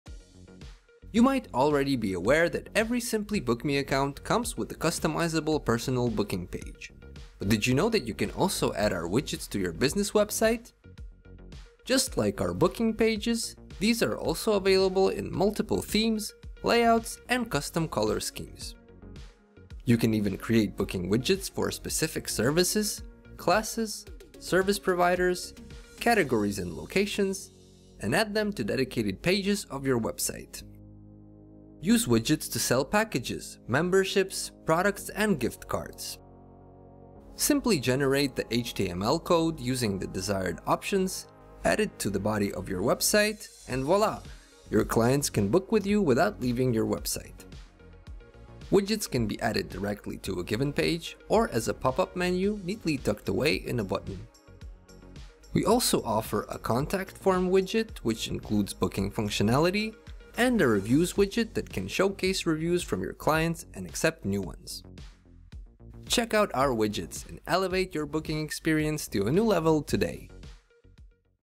Why you should use our Widgets
This video showcases why you should use our widgets to add booking widgets to pages of your personal website